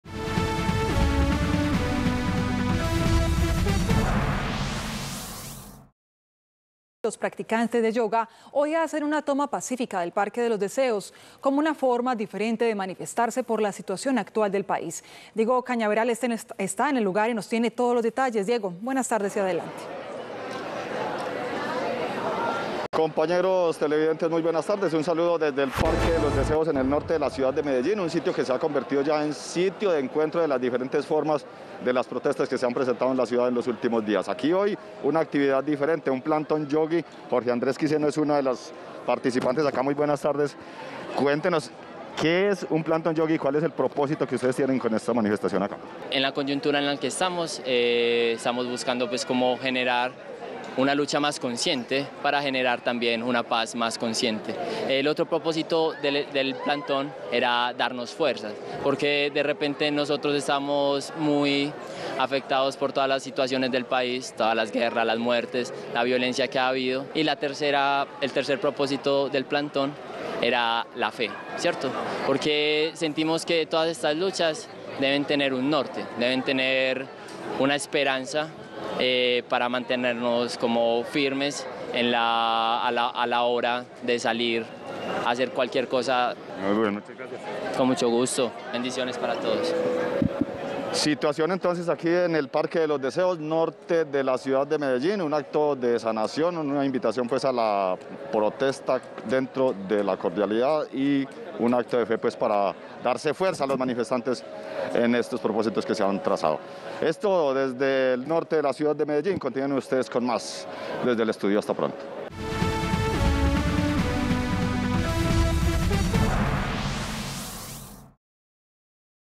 Manifestación a través del yoga - Teleantioquia Noticias
Los practicantes de yoga, hoy hacen una toma pacífica del Parque de los Deseos, como una forma diferente de manifestarse por la situación actual del país.